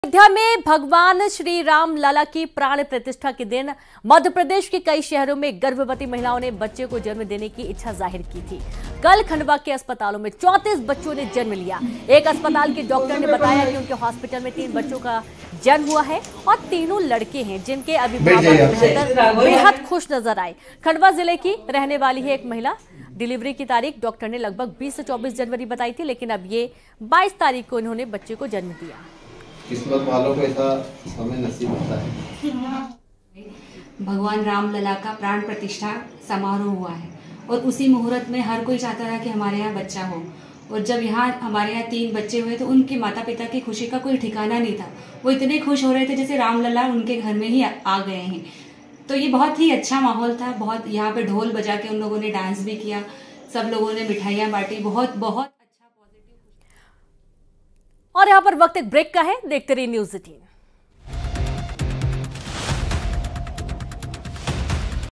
Ram Mandir : Shri Ram के आगमन पर Khandwa के Hospital में गूंजी किलकारी, परिजन बेहद खुश | Top News
Shri Ram के आगमन पर Khandwa के Hospital में गूंजी किलकारी, परिजन बेहद खुश